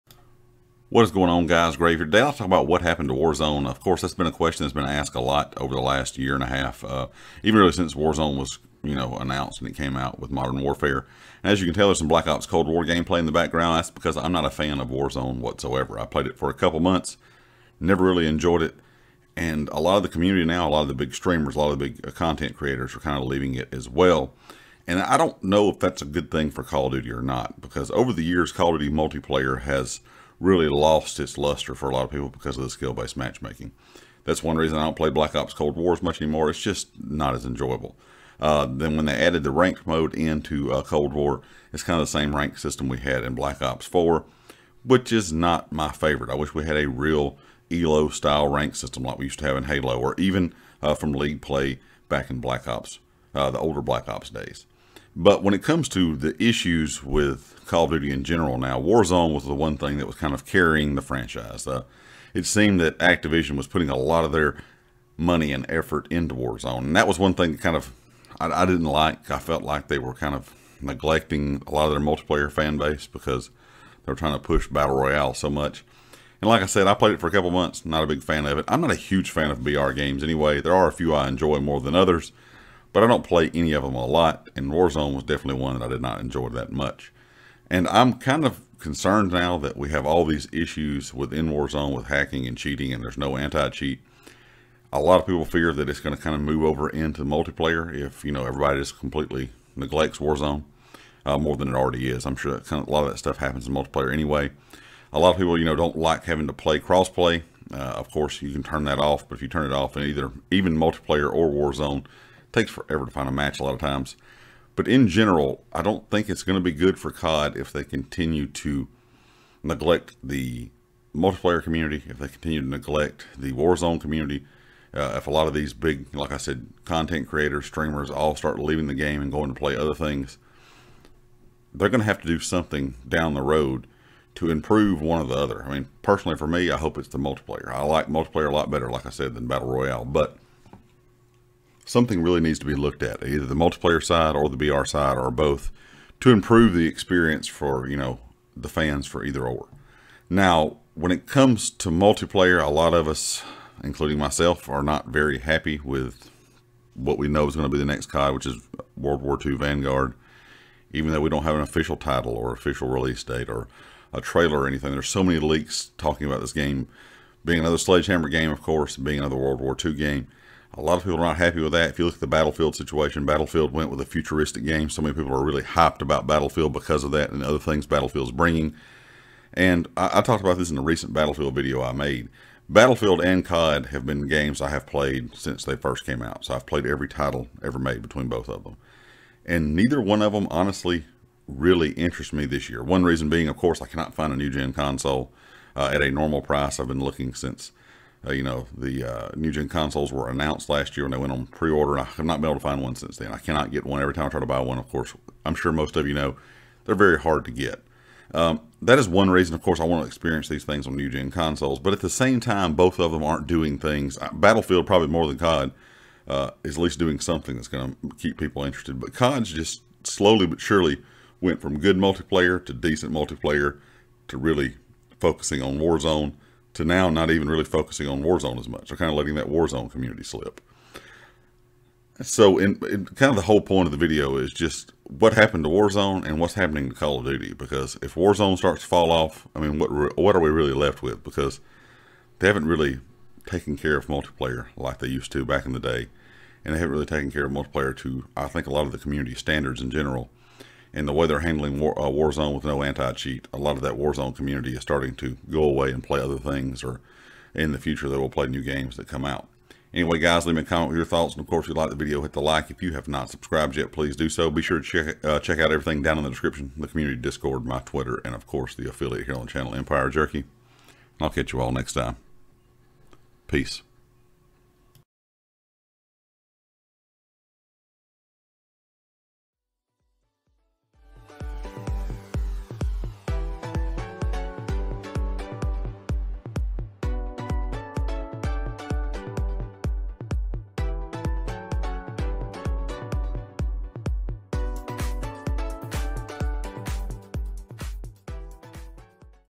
What Happened To Warzone? Bad News For Call of Duty
With all the issues going on in warzone and call of duty multiplayer what needs to be done to get the community excited about cod again? Thanks for watchin!